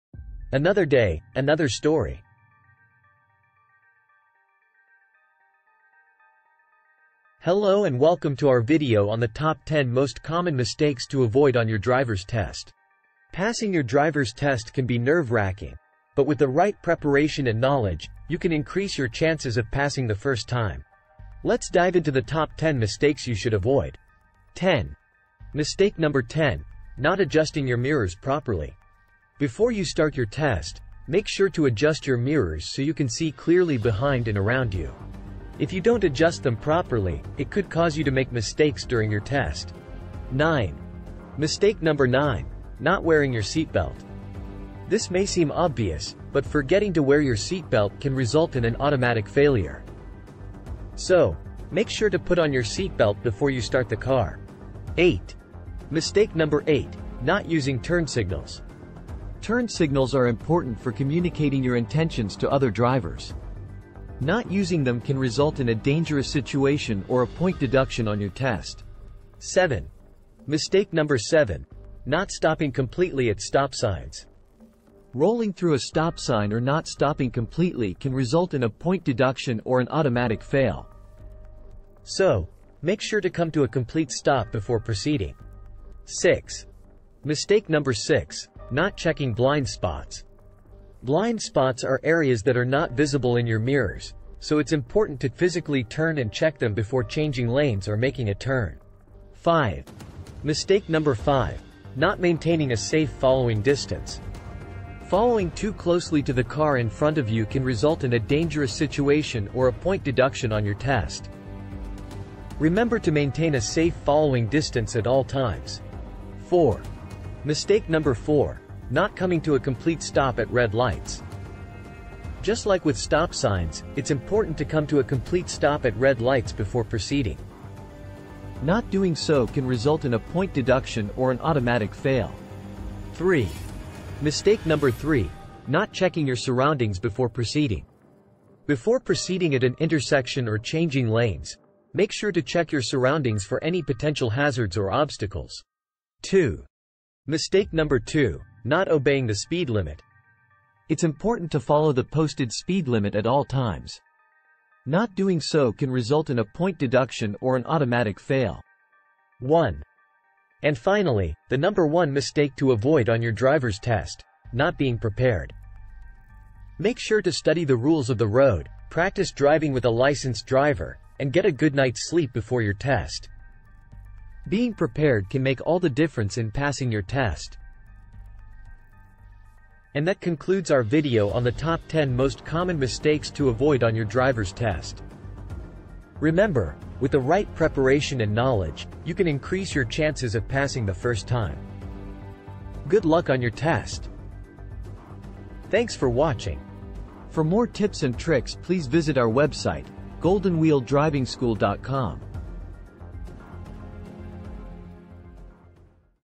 Top 10 Most Common Mistakes to Avoid on Your Driver's Test
This video is a comprehensive guide on the top 10 most common mistakes to avoid on your driver's test. The host discusses each mistake in detail and provides tips on how to avoid them. The video covers important topics such as adjusting mirrors, wearing seatbelts, using turn signals, stopping completely at stop signs and red lights, checking blind spots, maintaining a safe following distance, checking surroundings before proceeding, obeying the speed limit, and being prepared for the test. The video aims to help viewers pass their driver's test by providing them with valuable insights and knowledge on how to avoid common mistakes that can result in point deductions or automatic fail. It is a helpful resource for anyone who is preparing to take their driver's test, whether it be for the first time or as a refresher.